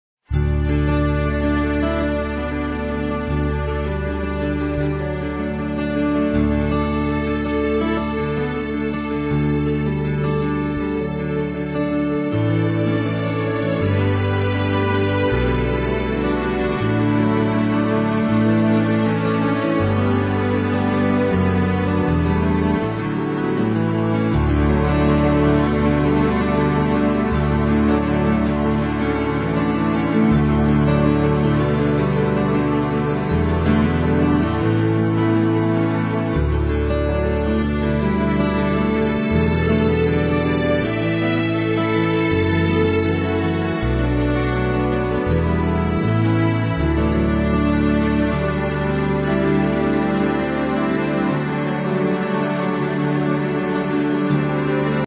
Ozark, MT Home For Sale - VirtuallyShow Tour #31241
THIS HOME RADIATES WARMTH, COMFORT, AND CHARM... Very nice 3 bedroom, 2 bath home with brick front, vinyl sides, tile floors in the living room, kitchen, baths, laundry and entry, wood trim, patio, inground sprinkler system. Large cul-de-sac lot.